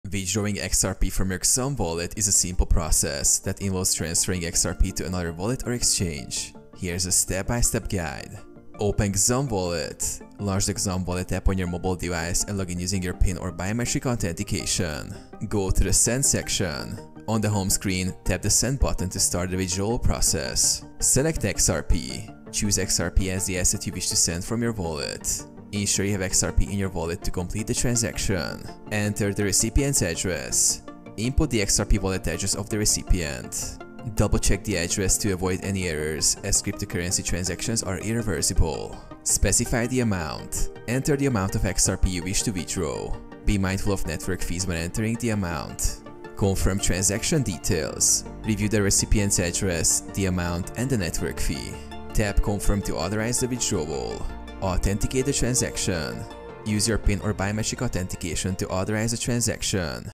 How To Withdraw XRP From Xumm Wallet (2025)
In this video I will explain How You Can Withdraw XRP From Xumm Wallet In 2025.